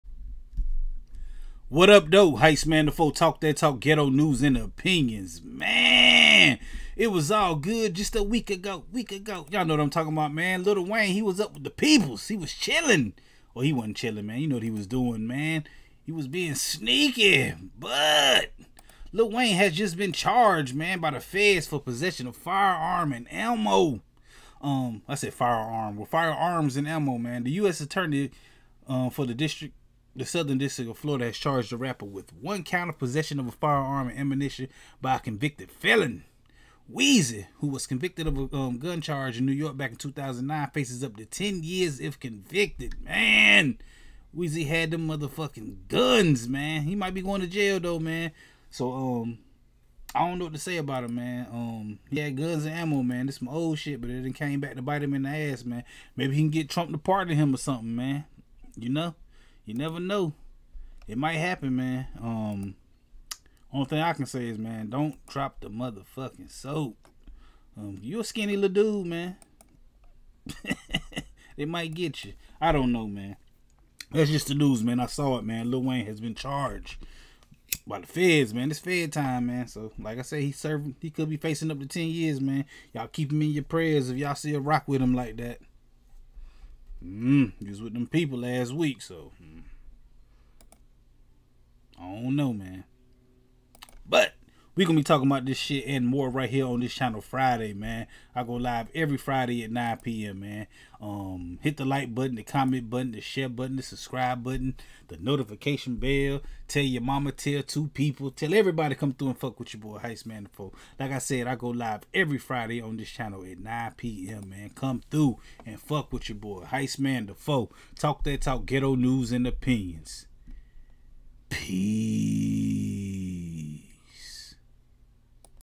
Lil Wayne CHARGED by Feds...Faces 10 YEARS, Possession Of Firearm and Ammo
Rapper Lil Wayne was charged by the feds for possession of firearm and ammo.